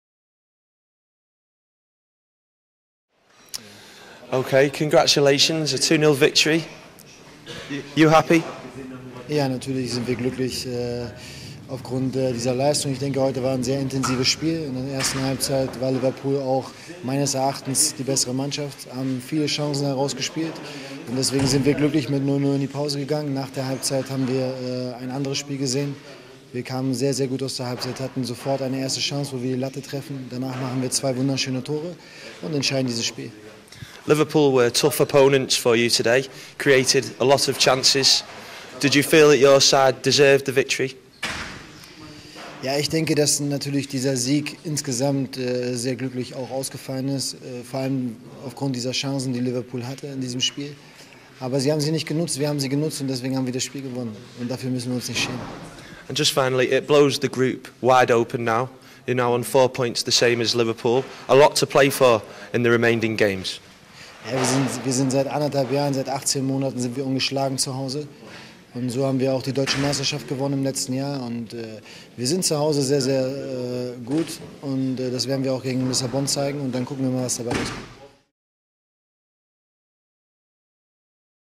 NextGen Series: Wolfsburg, interview
NextGen Series: Interview with Wolfsburg coach, after their 2-0 victory over Liverpool.